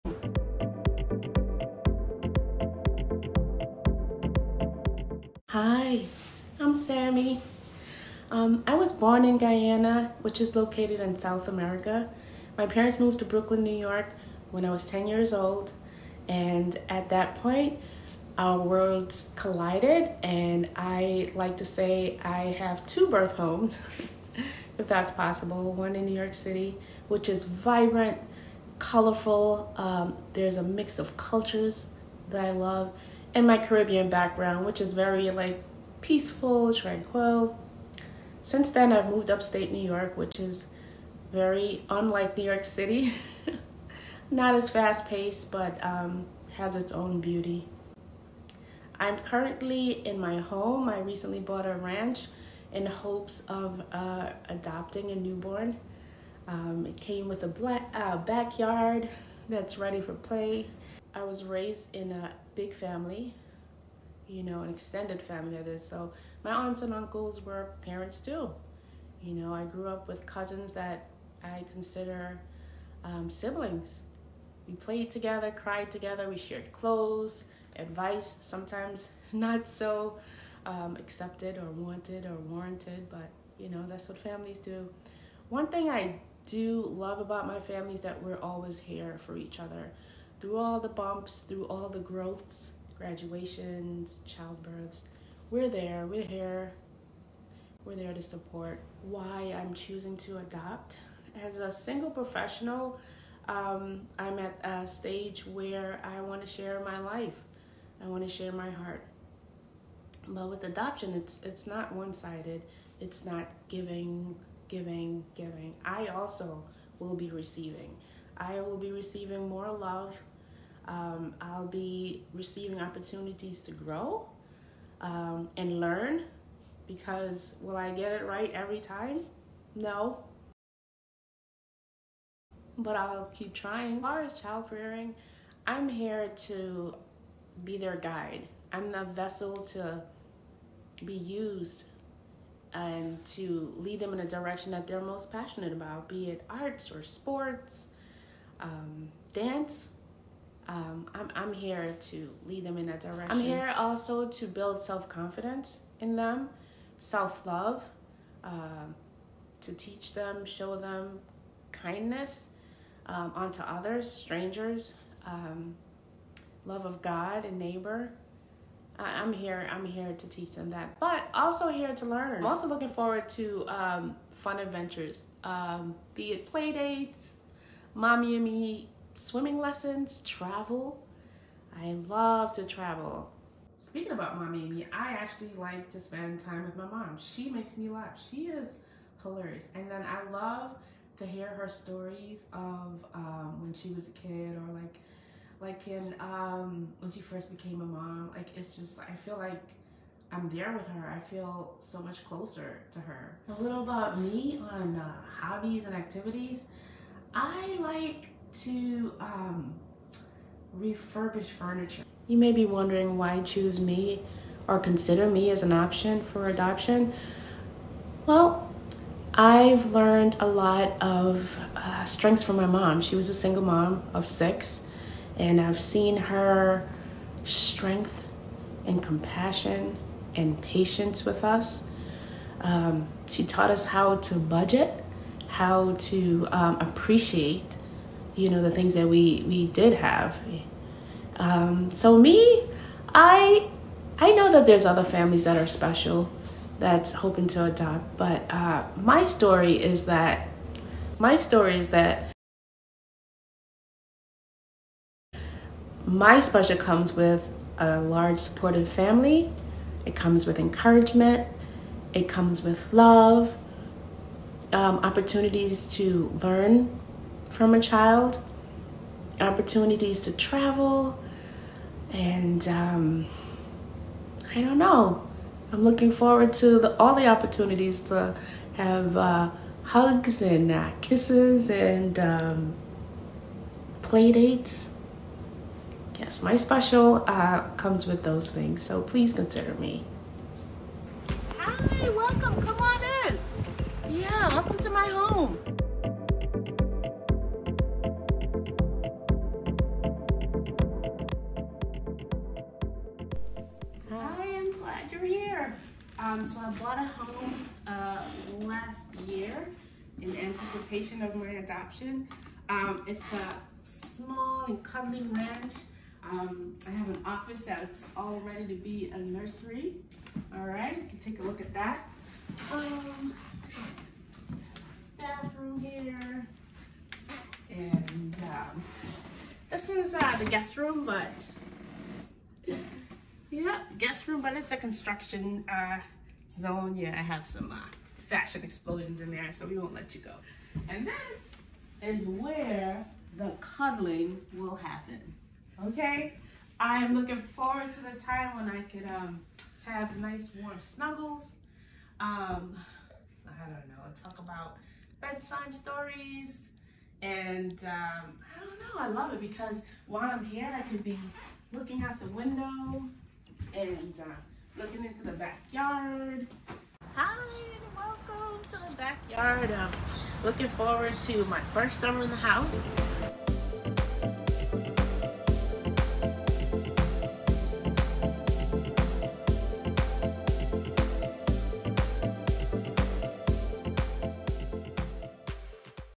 Sammie’s Hoping to Adopt
Hi, come along on my adoptionjourney. Get to know me & my support team better. Please reach out if you or someone you know is pregnant and has adoption as an option.